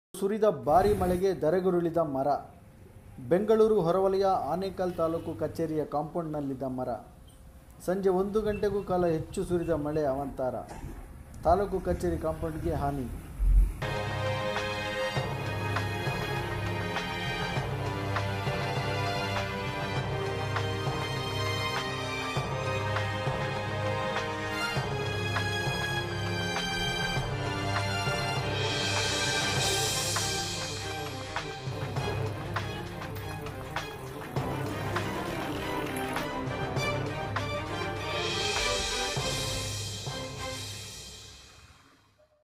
Veganews24
ಆನೇಕಲ್.

ಇಂದು ಸುರಿದ ಬಾರಿ ಮಳೆಗೆ ಧರೆಗುರುಳಿದ ಮರ.

ಬೆಂಗಳೂರು ಹೊರವಲಯ ಆನೇಕಲ್ ತಾಲ್ಲೂಕು ಕಚೇರಿಯ ಕಾಂಪೌಂಡ್ ನಲ್ಲಿದ್ದ ಮರ.

ಸಂಜೆ ಒಂದು ಗಂಟೆಗೂ ಹೆಚ್ಚು ಕಾಲ ಸುರಿದ ಮಳೆಯ ಅವಾಂತರ.

ತಾಲ್ಲೂಕು ಕಚೇರಿ ಕಾಂಪೌಂಡ್ ಗೆ ಹಾನಿ.